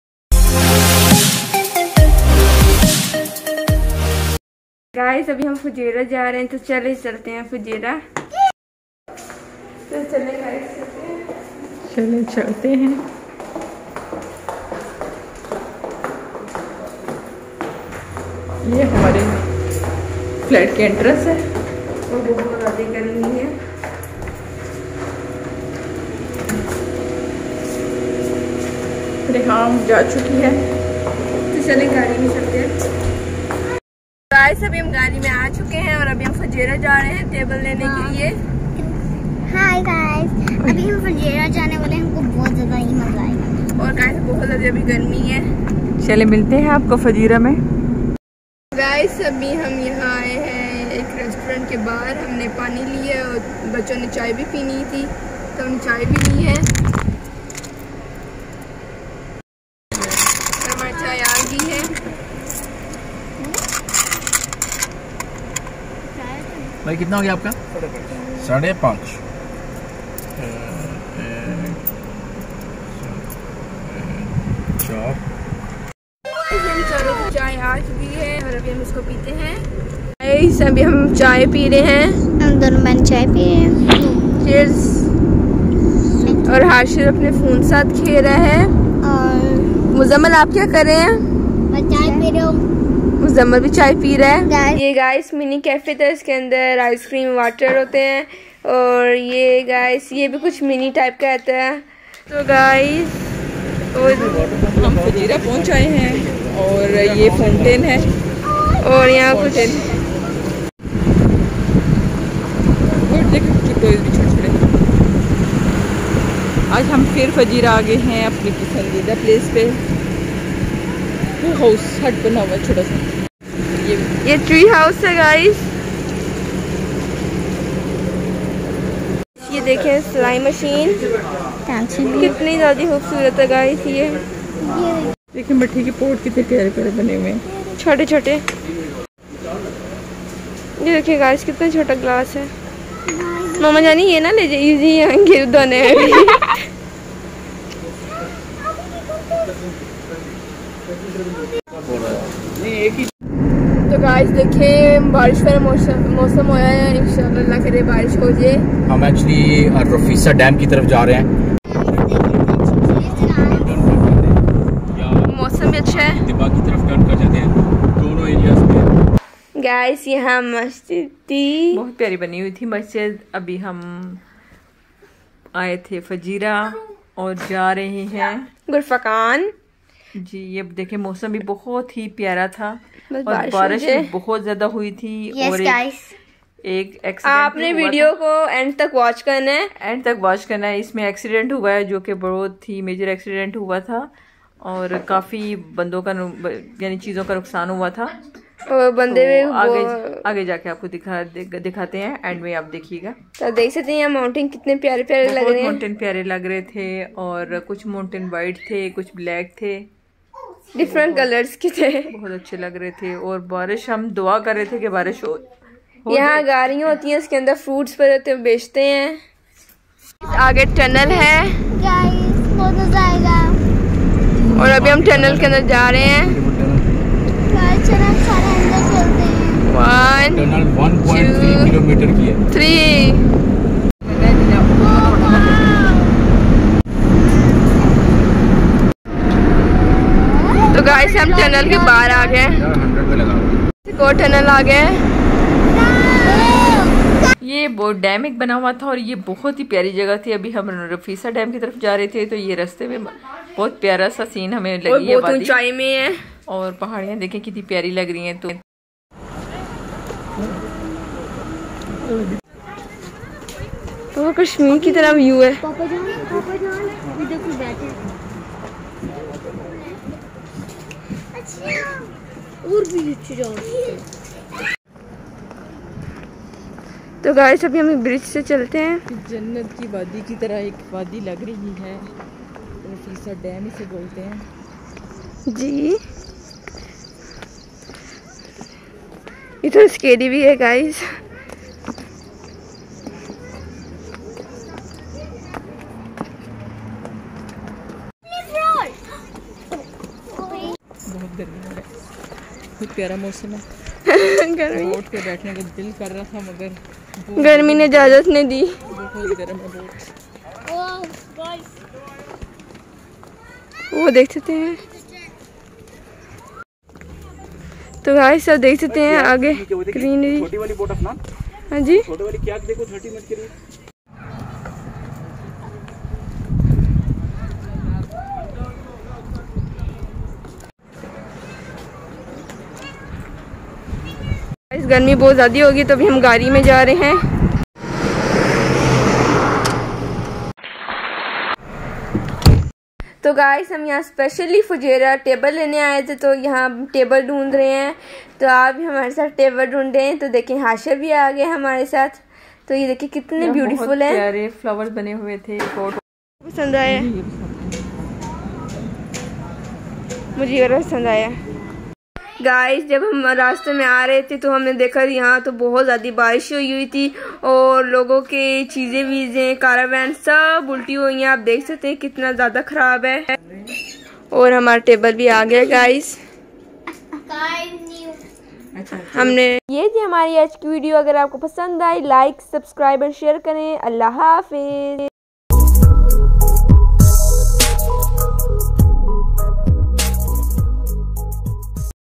HEAVY RAIN IN FUJEIRAH DESTROYED EVERYTHING || RAFISAH DAM KHOR FAKKAN || AFNAN WORLD ENTERTAINMENT
everyone today we went to Rafisah dam  which is in khor FAKKAN and the weather was so hot then after sometime it started to rain and there was several accident due to heavy rain and there was sand Storm we went to buy a table and we bought it at the end of the video so don't forget to like subscribe and share if you're new to our channel thanks for watching stay connected ☺️🙂